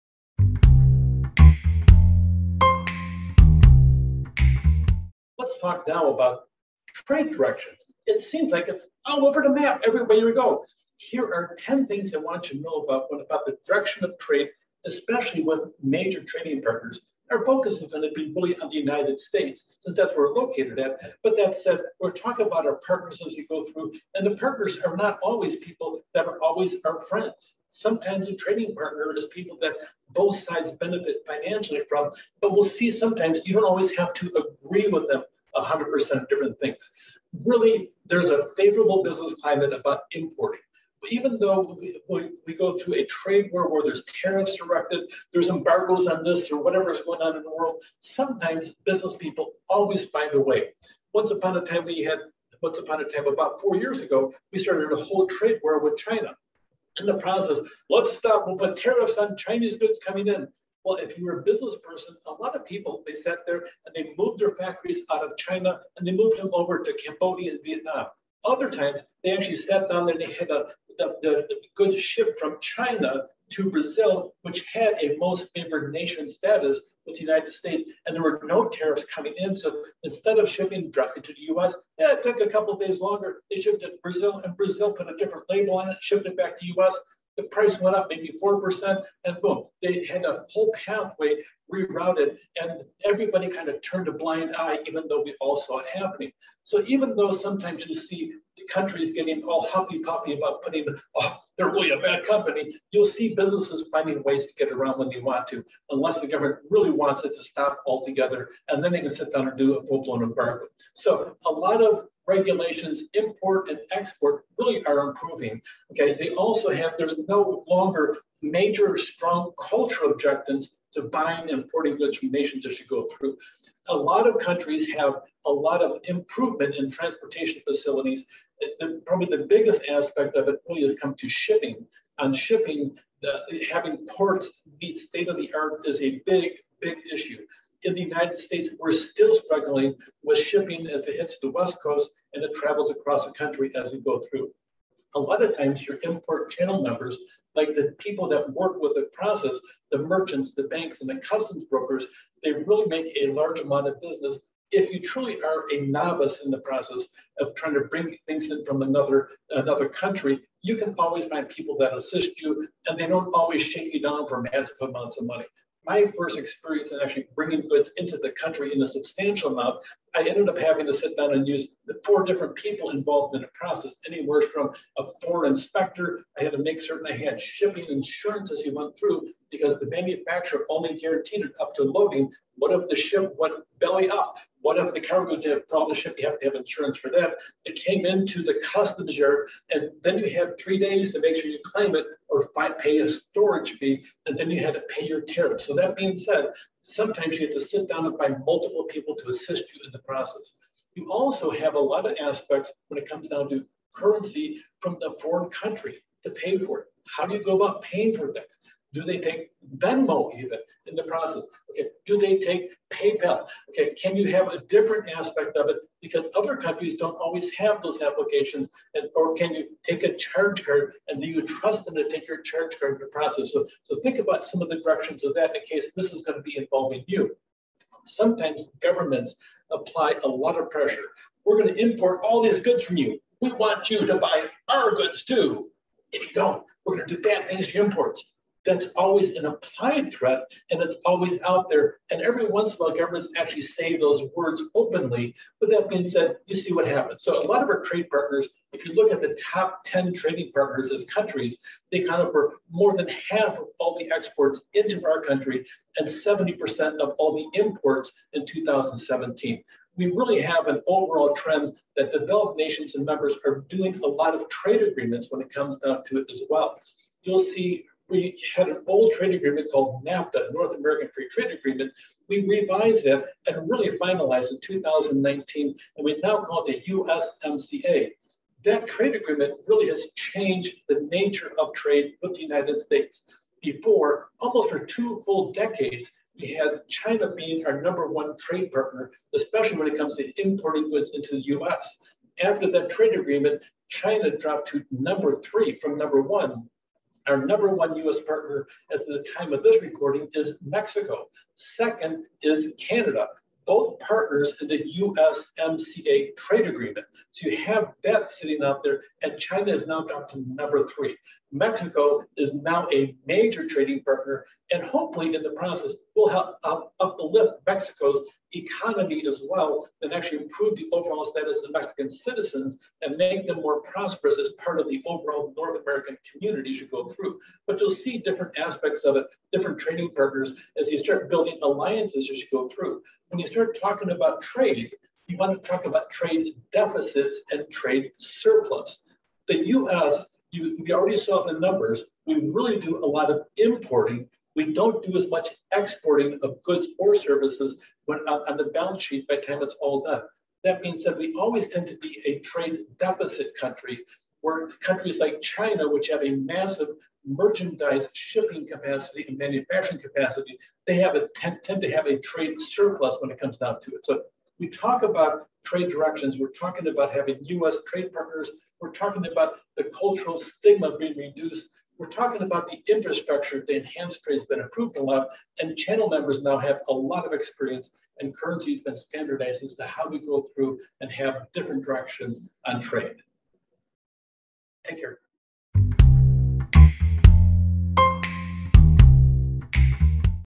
Trade Directions: Int'lBus C2
The direction of international trade has been particularly favorable to the partners of the United States. As the USA shipped manufacturing overseas and became  a service economy, the countries which became the homes of manufacturing thrive and send the business, we initially provided through foreign direct investment, back to our shores as imports. Sometimes the export industry changes into the import industry through the International Product Life Cycle!